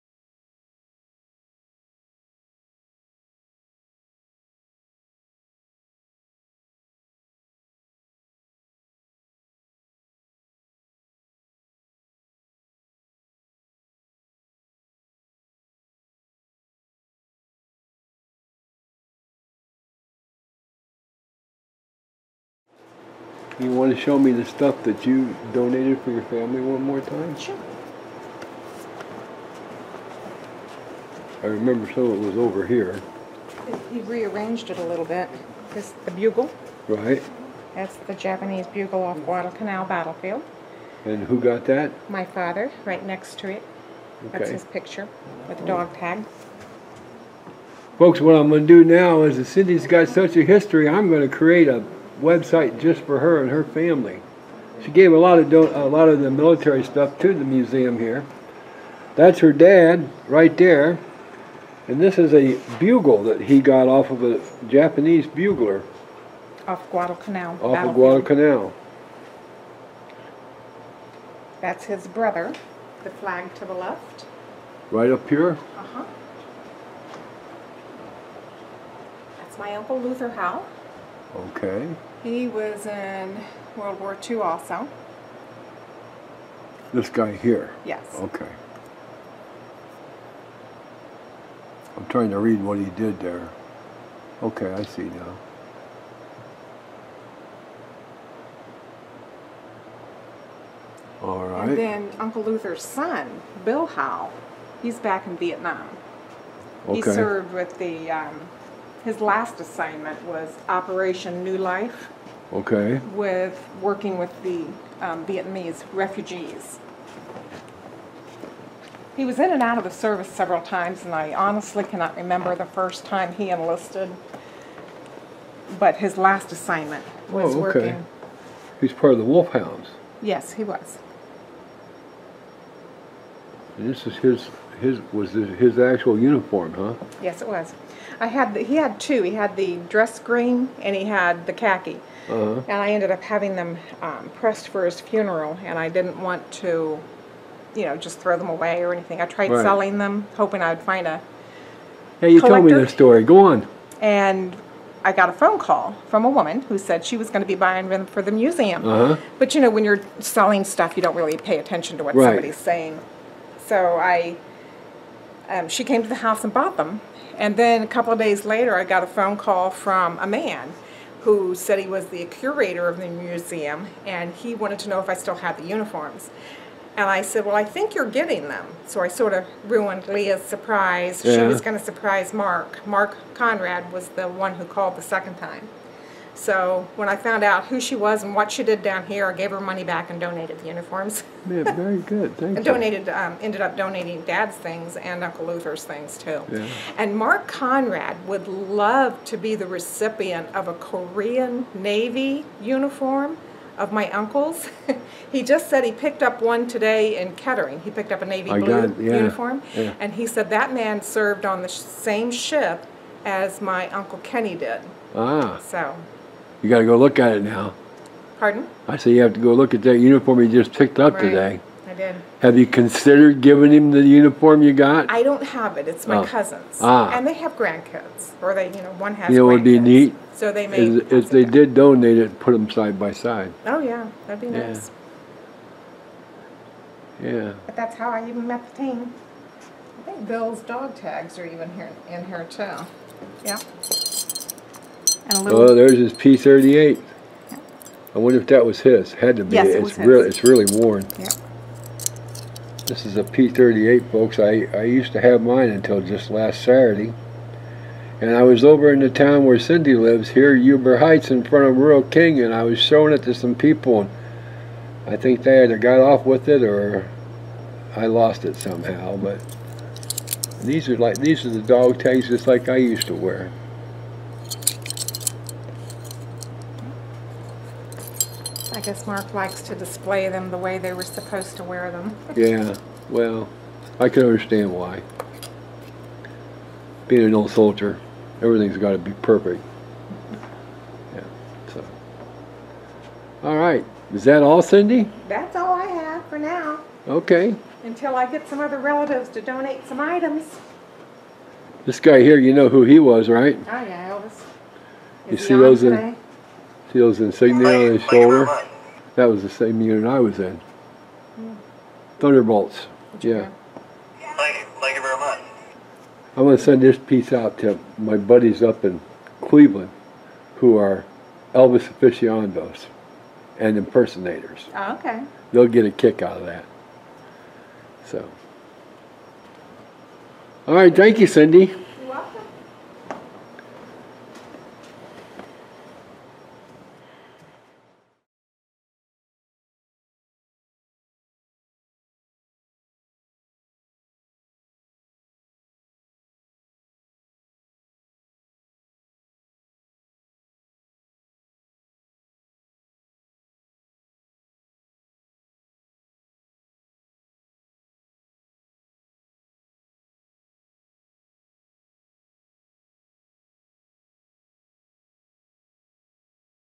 Howell Family Has a long history of Military.
Cyndy Howell Sloan shares the history of the military in her family with us.  Thank You Cyndy for the sacrifices you made bringing this to share with the world.